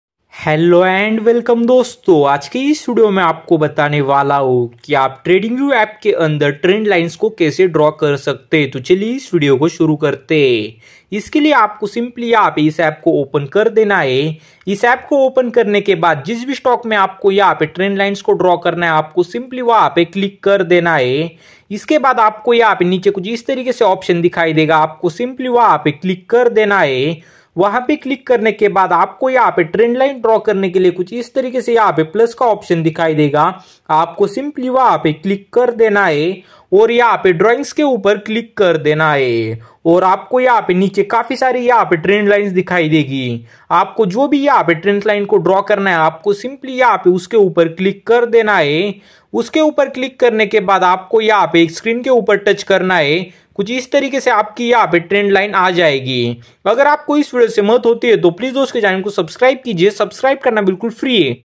how to draw trend line in tradingview in mobile
This video is about how to draw trend lines in trading view mobile app